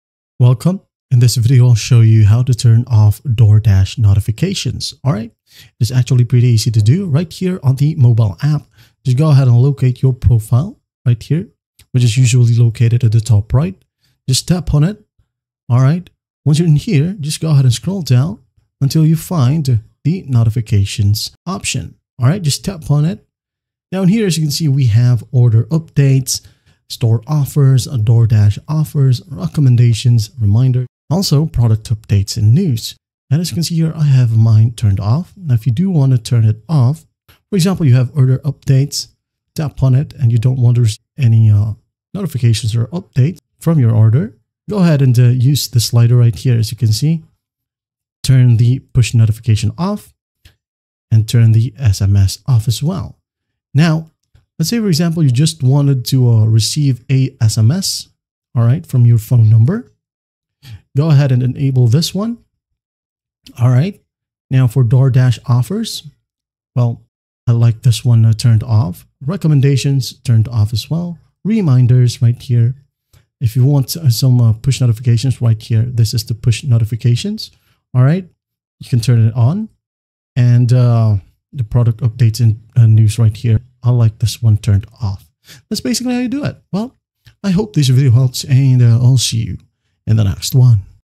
How to TURN OFF DoorDash Notifications (Step by Step)
Tired of constant alerts from the DoorDash app? This step-by-step tutorial shows you how to disable DoorDash notifications on both iPhone and Android devices. You'll learn how to manage push alerts, promotional messages, and order updates to customize your notification preferences. Whether you're a customer or a Dasher, this guide helps you take control of your app experience.